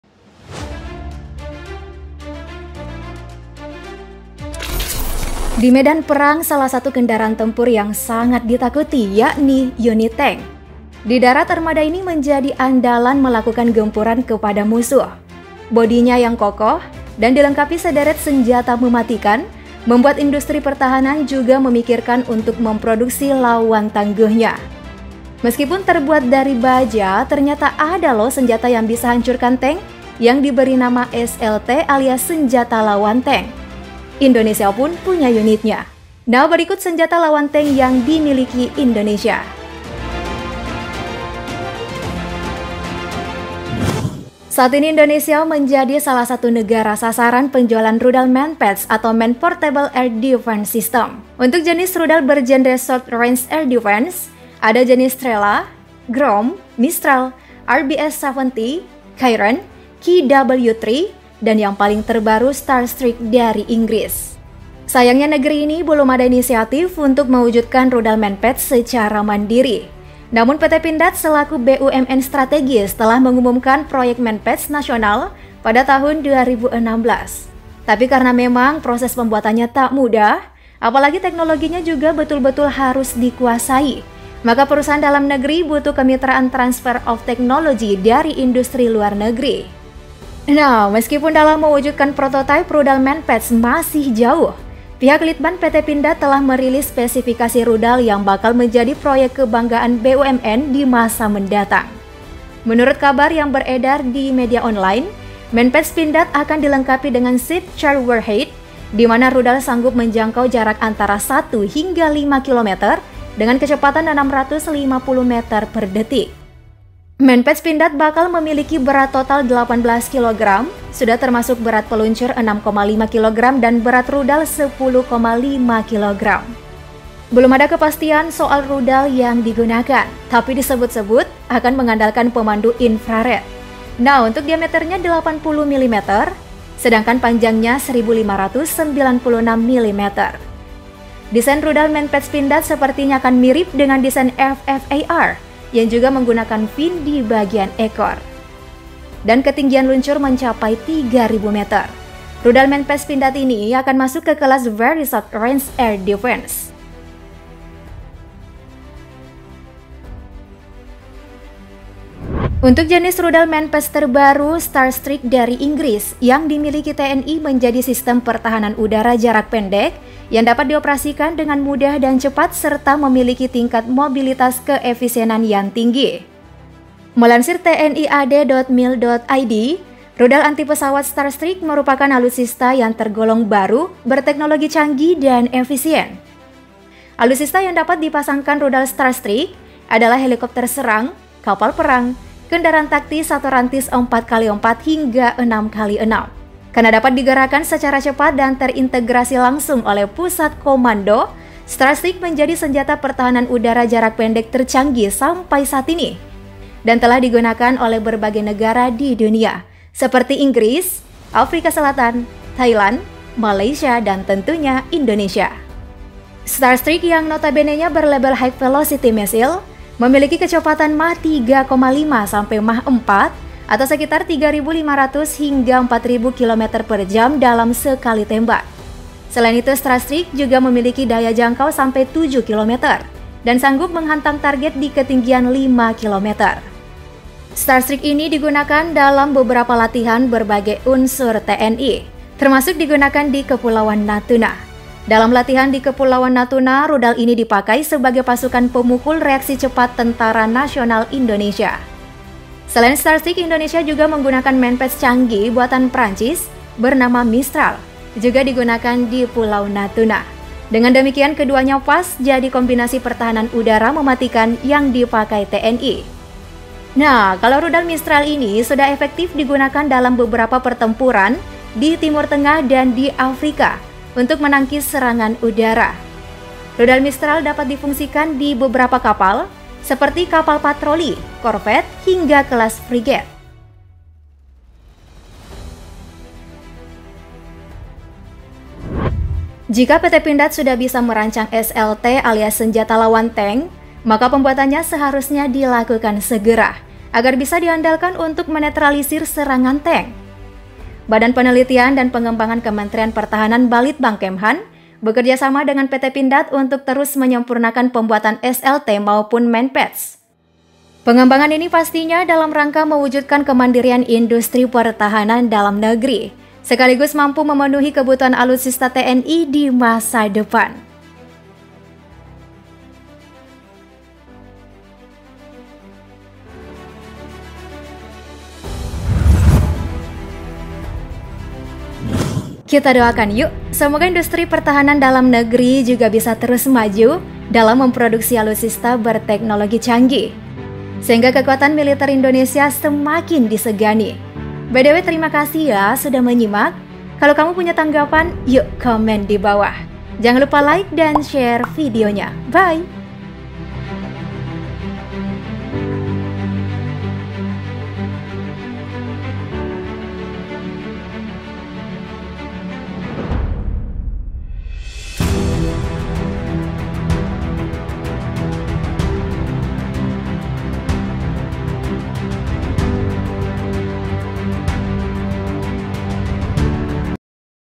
BAKAL JADI ANDALAN TNI DALAM MEMPERTAHANKAN NKRI! Inilah Senjata Lawan Tank yang Dimiliki Indonesia
Di medan perang, salah satu kendaraan tempur yang sangat ditakuti, yakni unit tank. Di darat, armada ini menjadi andalan melakukan gempuran kepada musuh. Bodinya yang kokoh dan dilengkapi sederet senjata mematikan, membuat industri pertahanan juga memikirkan untuk memproduksi lawan tangguhnya. Meskipun terbuat dari baja, ternyata ada loh senjata yang bisa hancurkan tank, yang diberi nama SLT alias Senjata Lawan Tank. Indonesia pun punya unitnya. Nah berikut, Senjata Lawan Tank yang Dimiliki Indonesia